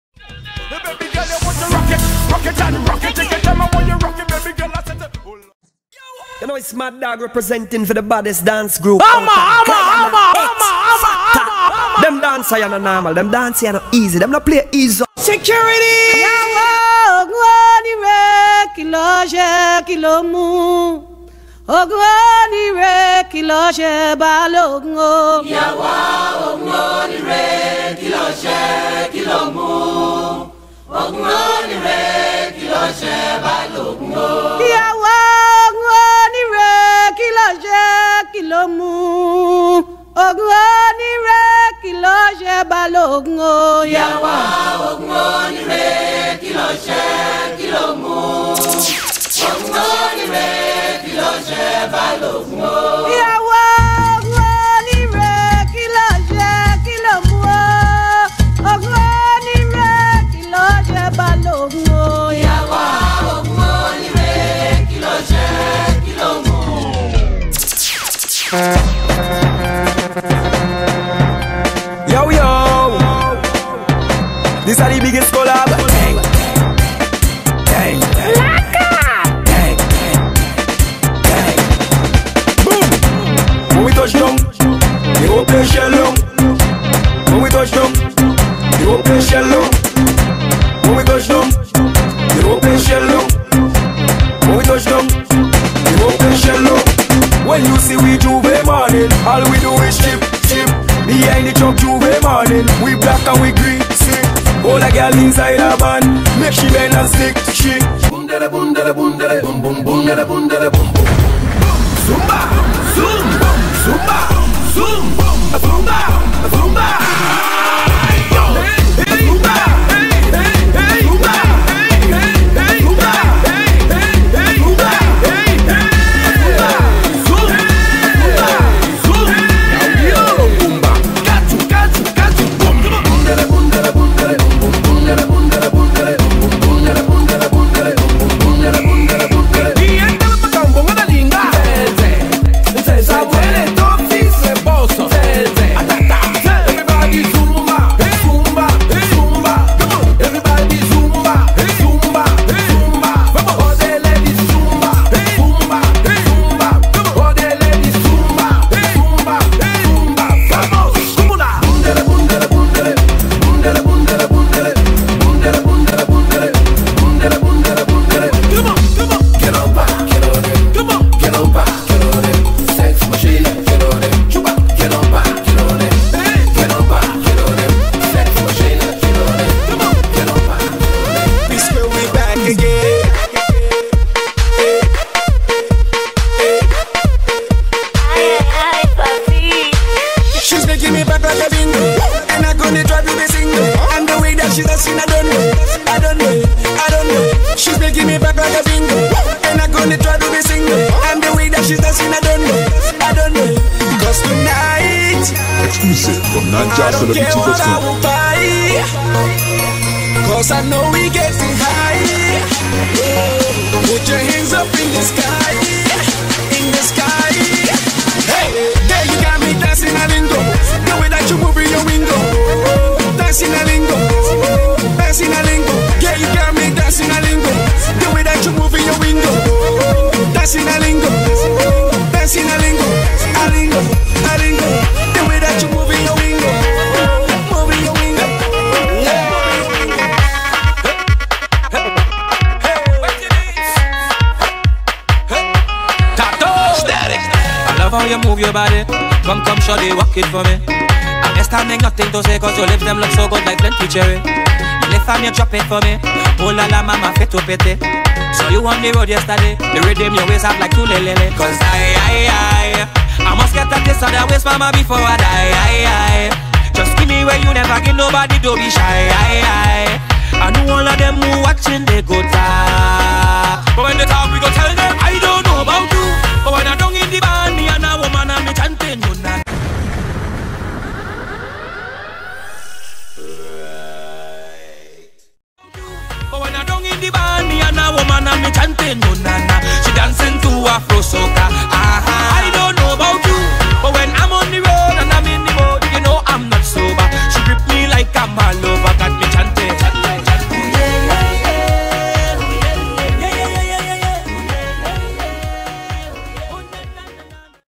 X FACTOR EMANCIPATION DAY
X FACTOR performing at the National Park, Thomas Lands, Georgetown, Guyana